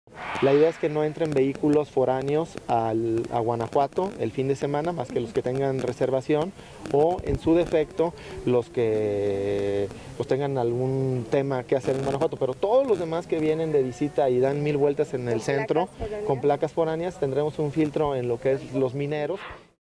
Solo personas con reservación de hotel podrán ingresar con su vehículo a Guanajuato
Alejandro Navarro, Presidente Municipal de Guanajuato vuelve a la carga contra las personas no originarias de esa ciudad. Ahora advierte que todos aquellos turistas que pretendan visitar el Festival Cervantino y no cuenten con una reservación de hotel la zona, no podrán ingresar Fue claro, al señalar que, esencialmente los fines de semana, se colocarán retenes en la entrada a la ciudad, a efecto de no permitir el paso de los autos con placas foráneas que no demuestren de manera fehaciente que previamente han realizado una reservación formal para instalarse en alguno de los hoteles que ahí operan.
En 2018 Alejandro Navarro Saldaña acusó que turistas que visitan esa parte de México, especialmente los provenientes de Aguascalientes, Morelia y Guadalajara compran viajes por 400 pesos, “no gastan”, llevan lonche, no consumen en sus restaurantes, generan basura, problemas de movilidad y “desgastan” la ciudad.